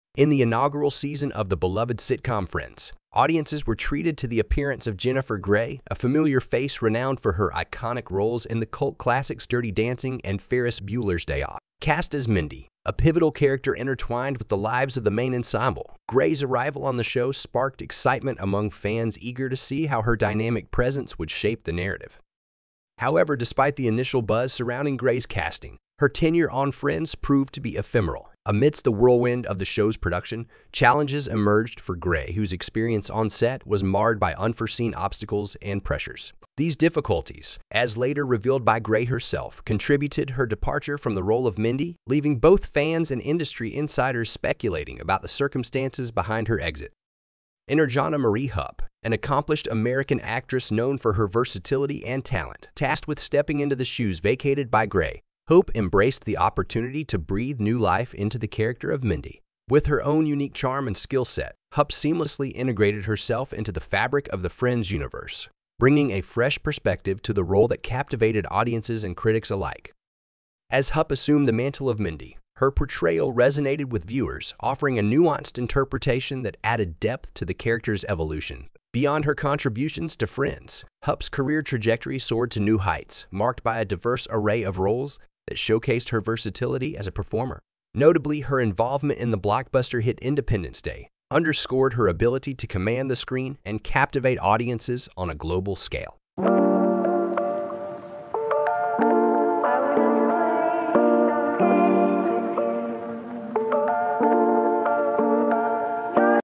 The Story Behind Jennifer Grey's Exit from Friends
Explore the intriguing tale of Jennifer Grey's brief stint on Friends as Mindy and the surprising replacement by Jana Marie Hupp. Uncover the challenges Grey faced on set and Hupp's subsequent success, including a role in Independence Day. Dive into the behind-the-scenes drama of one of the most iconic sitcoms of the '90s!